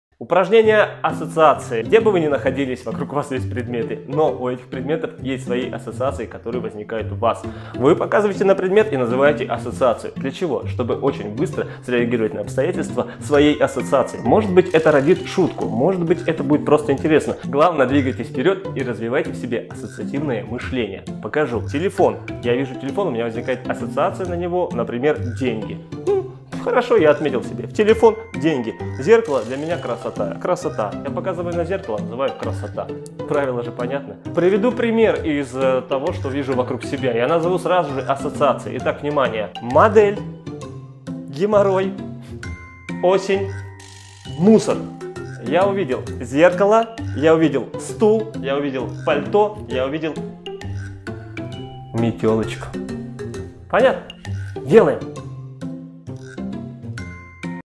Словесная импровизация. Упражнение "Ассоциации" ver.1
Серия видео уроков для самостоятельного обучения.
Подходит для поддержания формы.
Если оно Вам кажется очень простым, увеличивайте скорость!
Да прибудет с Вами